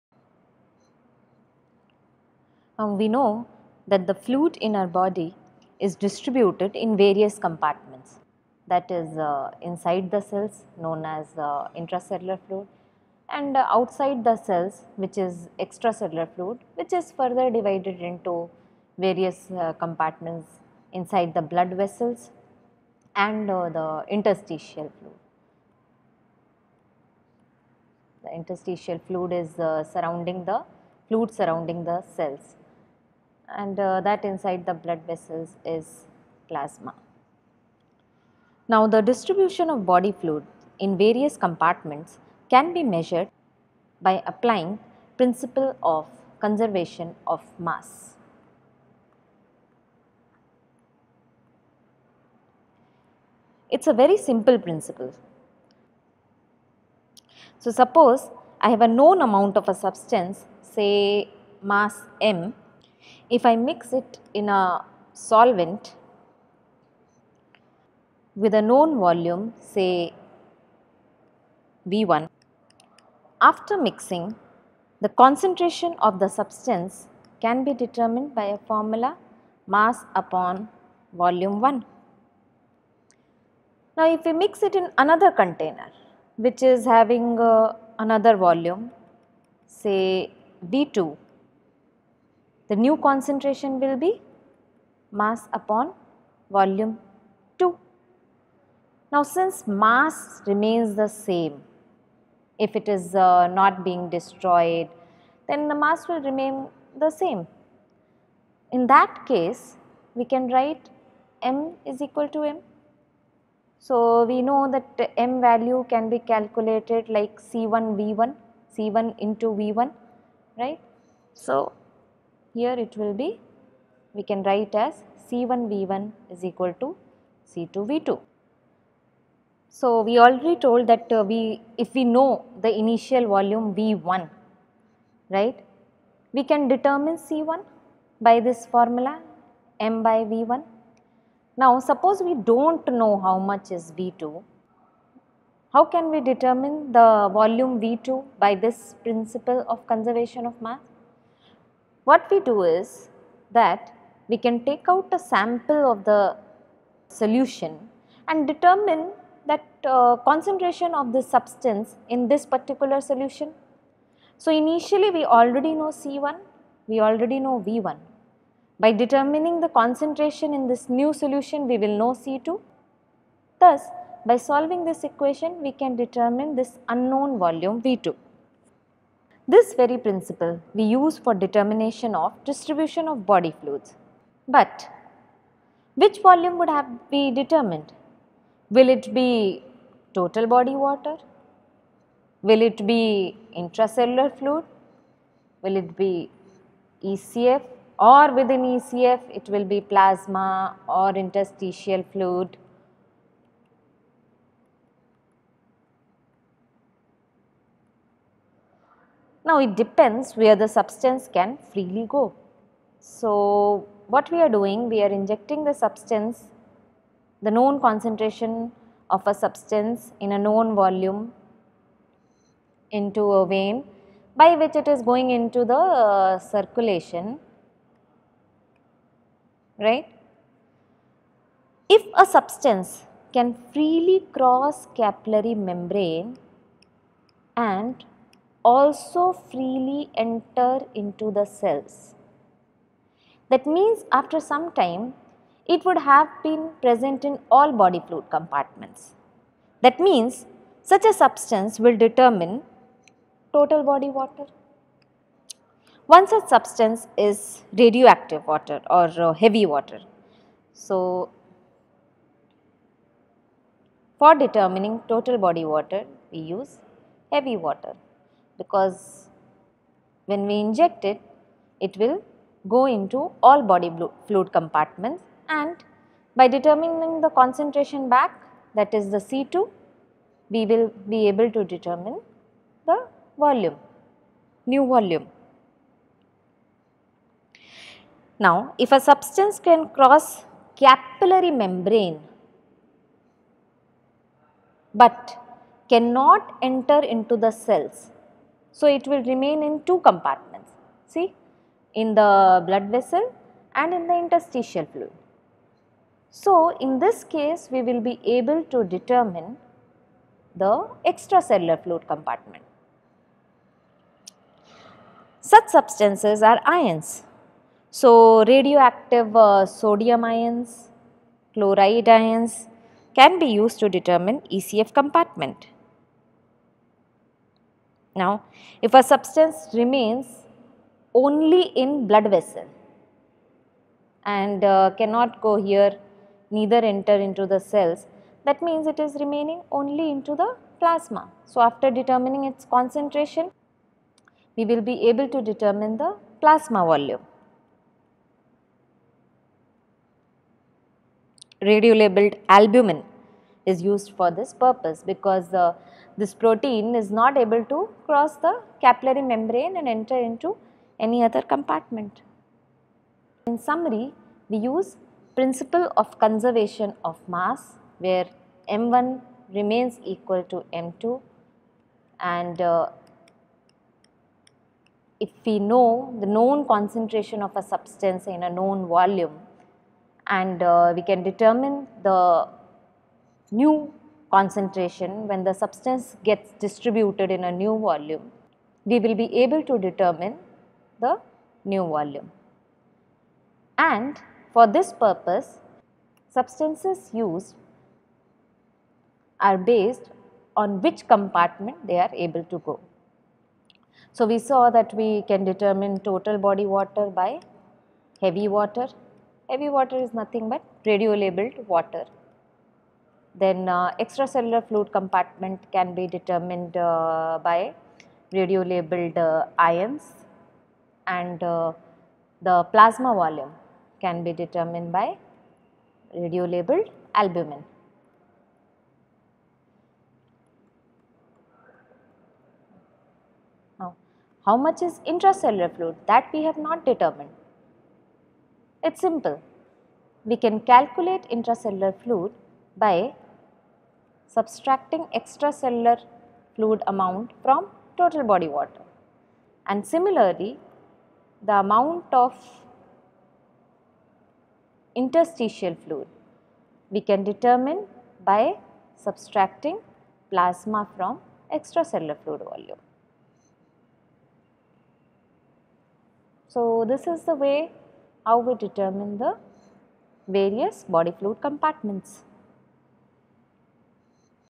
Body fluid compartments and measurement | General Physiology mbbs 1st year lecture
Physiology lecture on measurement of body fluid compartments explains body fluid compartments, principle of measurement of body fluids and the substances used.

What are the various body fluid compartments and their percentage water distribution? Total body water is 60% of body weight and is divided into various body fluid compartments. These include intracellular fluid, and extracellular fluid. Extracellular fluid compartment further consists of Interstital fluid and plasma. The video further explains the the principle of measurement of body fluid compartments? What is the principle of conservation of mass?
What are the substances used to measure various body fluid compartments?

Chapters:
00:00 Body fluid compartments
00:38 Principle of measurement of body fluids
03:37 Substance for measuring various body fluid compartments